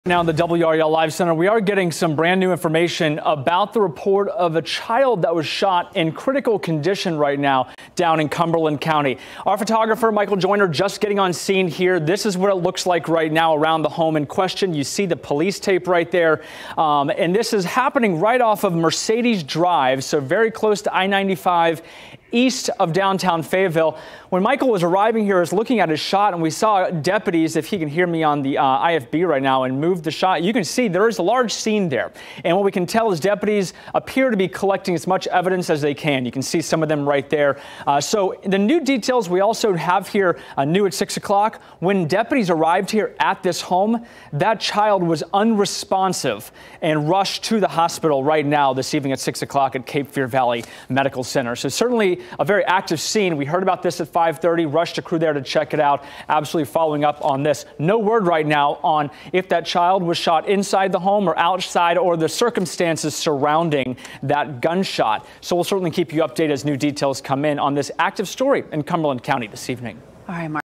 Child shot in Fayetteville on Mercedes Drive
A child was taken to the hospital Tuesday afternoon with a gunshot wound.